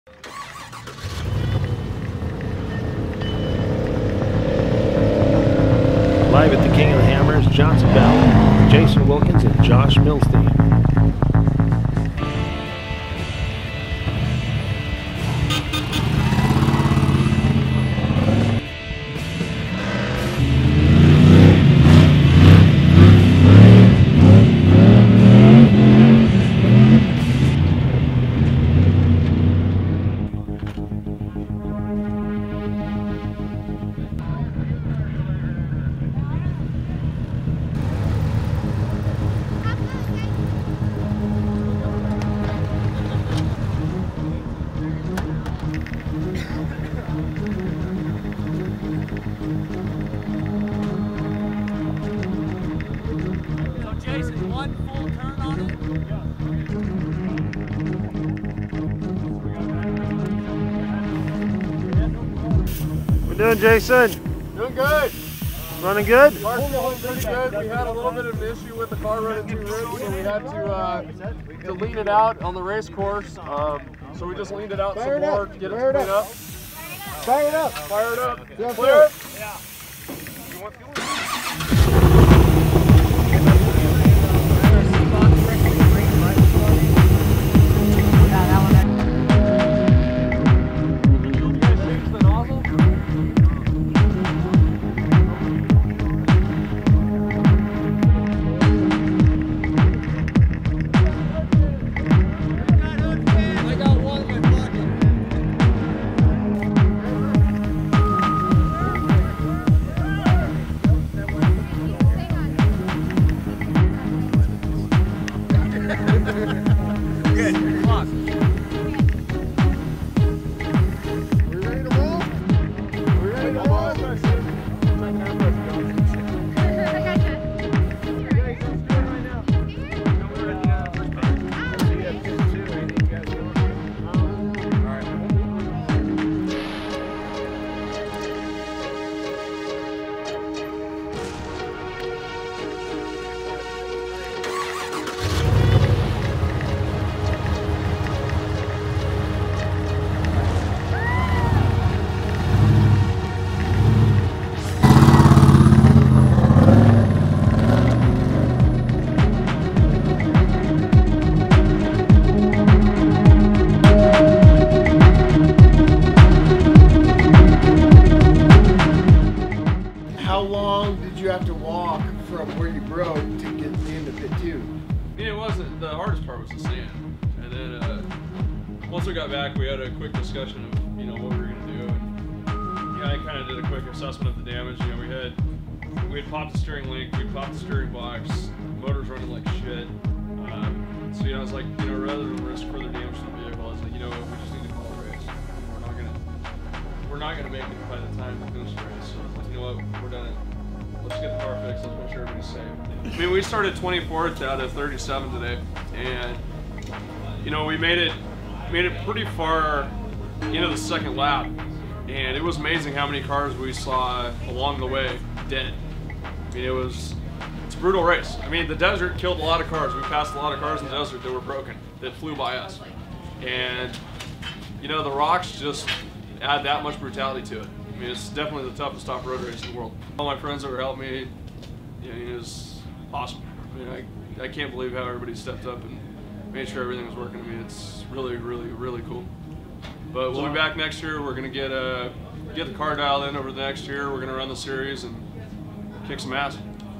King of the Hammers KOH 2014 Race Car #4596
Jason Wilkins and josh Milstein racing at the King of the hammers - Smittybilt  Everyman Challenge  Race Car #4596 Short clips.  Johnson Valley California.